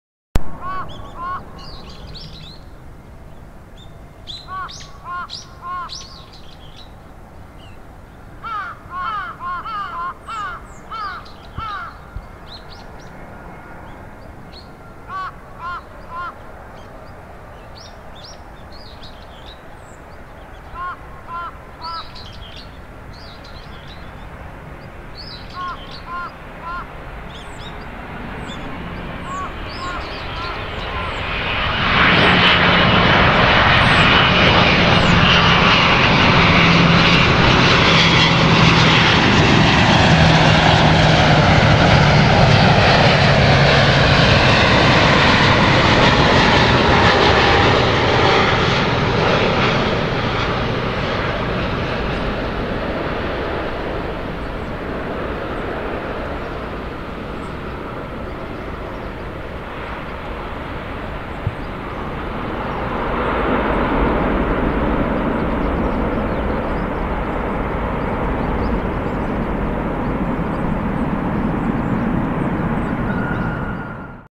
Alitalia Boeing B777 landing in fog @Narita Airport rwy 34L
成田空港畑ポイントにて。
■movie camera : JVC Everio Gz-E565
■IC recorder : sony ICD-UX523
■microphone : Audio technica AT9913